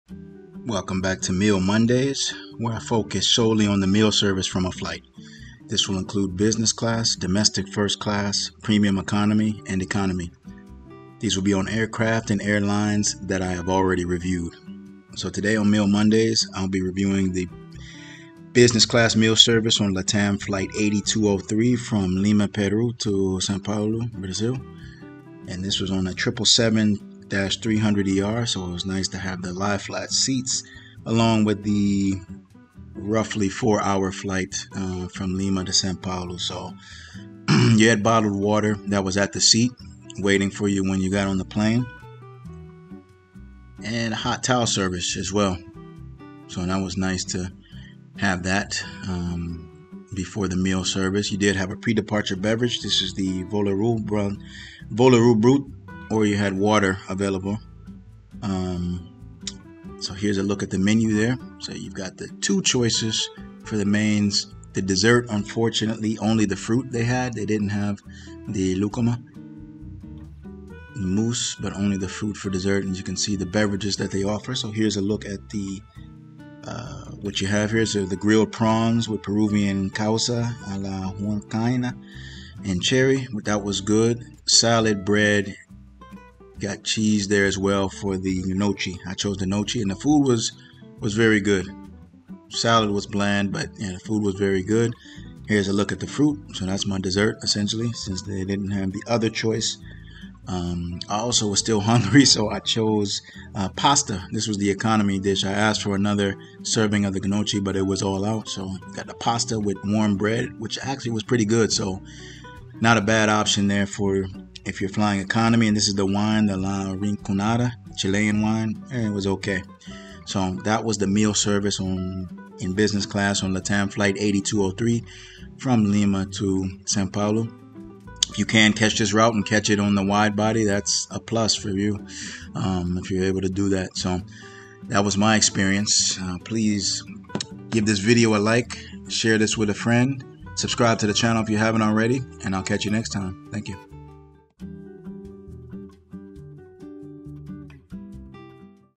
Meal Monday's - LATAM Meal Service LIM-GRU Business Class
Today we look at the meal service in LATAM's Business Class from LIM-GRU.